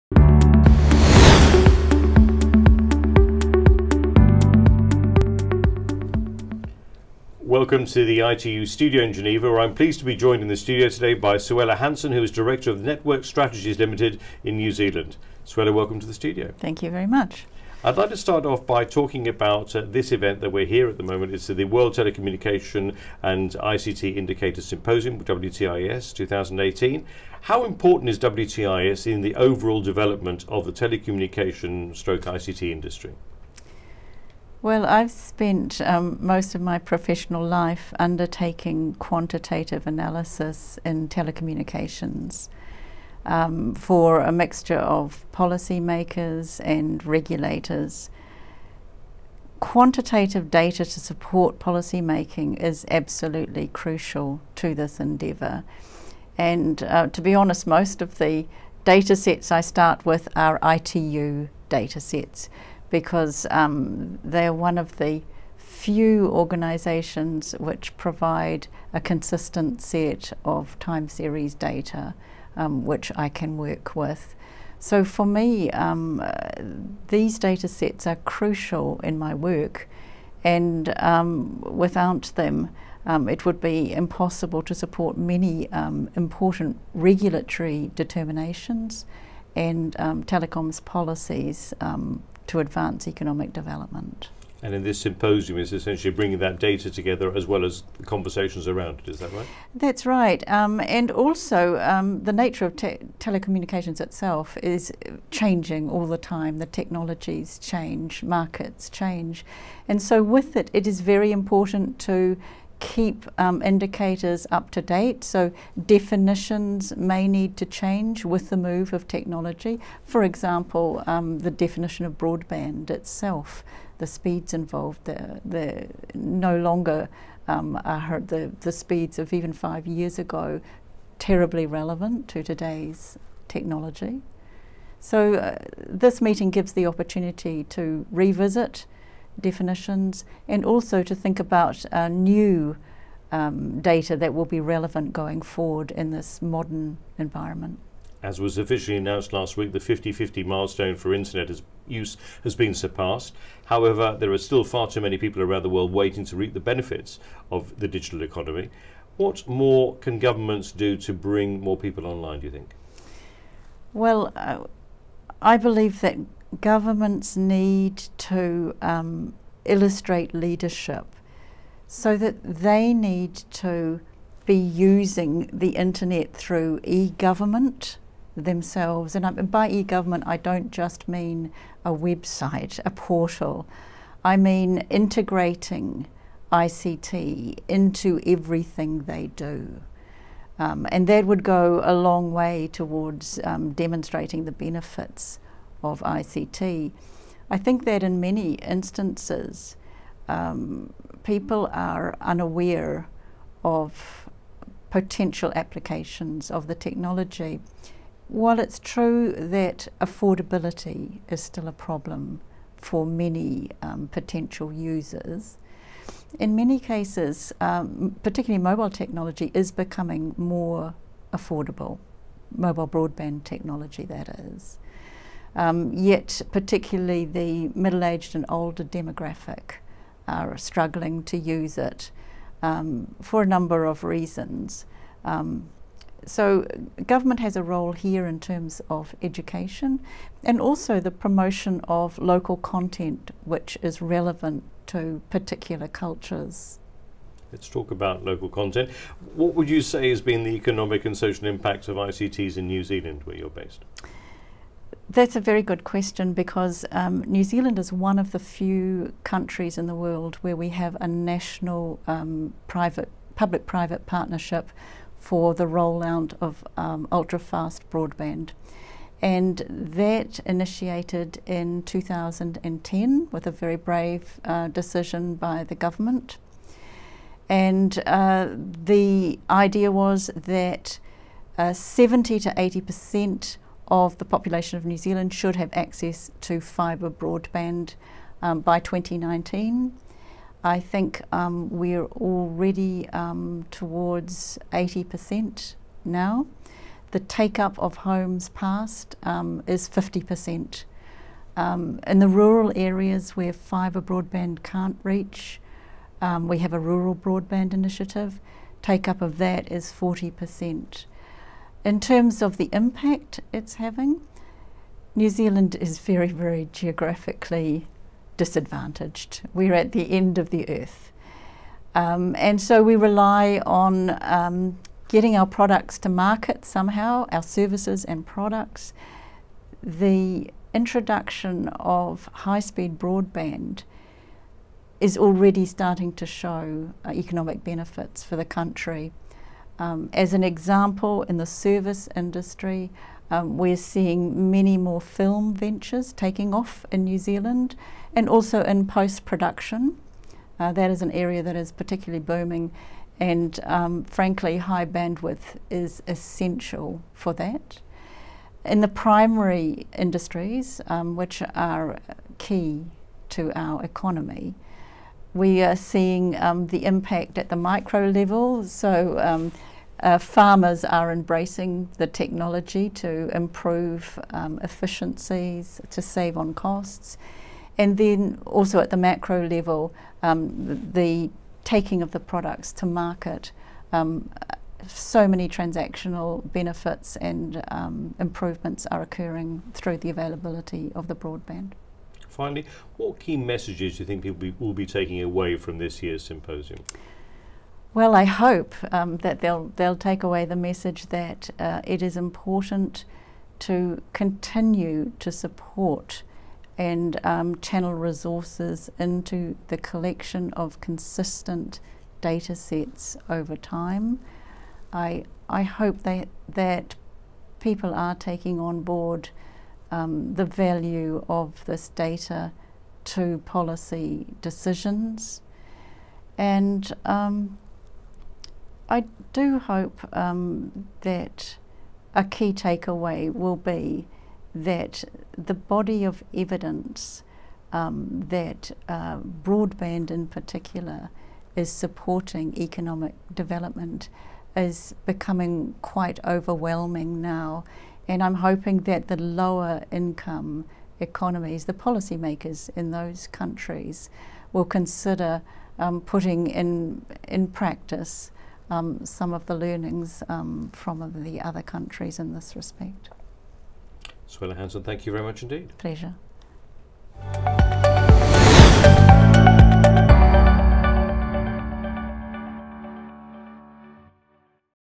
ITU INTERVIEWS @ WTIS-18: Suella Hansen, Telecommunications Economist, New Zealand
Interview with Suella Hansen, Telecommunications Economist, New Zealand

WTIS-18 features high-level debates on the impact of telecommunications/ICTs and emerging technologies on social and economic development. Other sessions of WTIS-18 will focus on measurement in an environment of artificial intelligence, big data, robotics and the Internet of Things. The work of the Expert Group on Telecommunication/ICT Indicators (EGTI) and the Expert Group on Household Indicators (EGH) will also be presented for adoption by WTIS-18. The Symposium also looks at how to measure ICT skills and how progress can be measured on implementing the Sustainable Development Goals (SDGs).